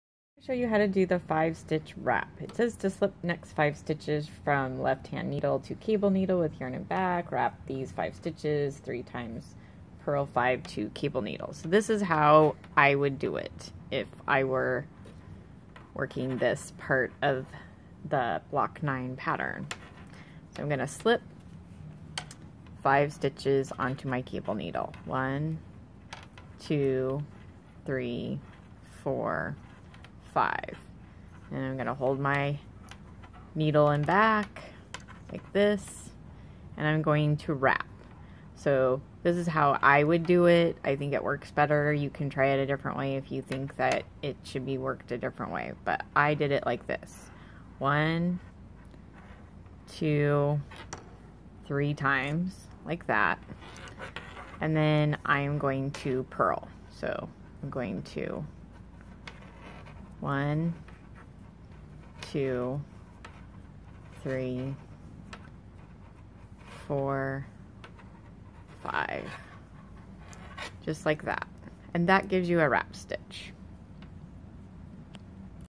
Wrap Stitch (for leaves) - Knitterati Afghan - Block 9
5-st WRAP Slip next 5 sts from LH needle to cable needle with yarn in back, wrap these 5 sts 3 times, p5 from cn.